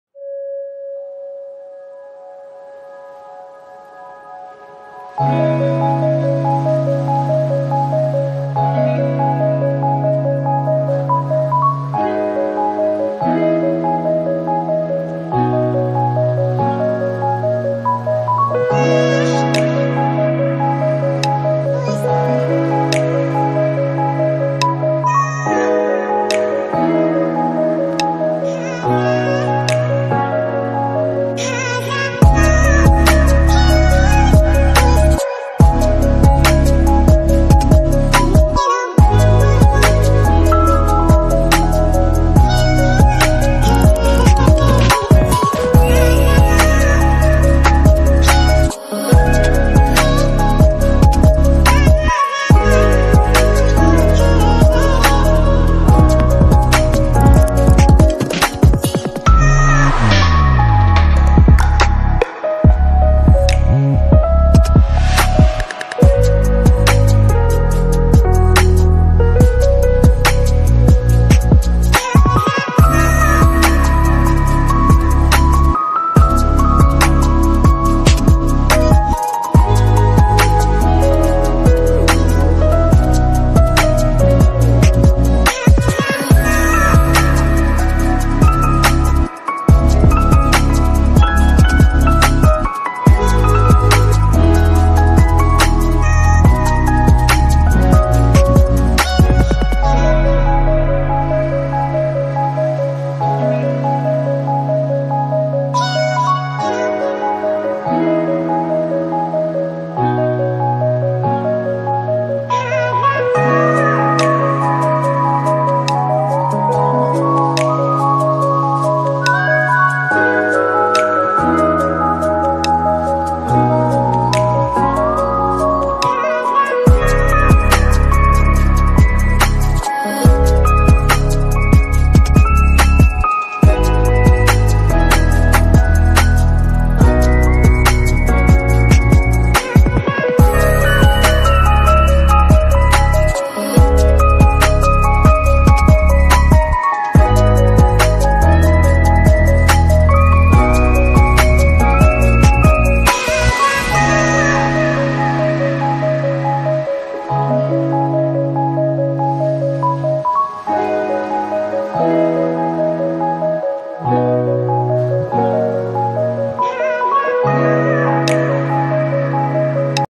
Pray for Australia🙏
The fire of Australia is can't stoped almost 50 million animals are dead in the cause so please pray for Australia.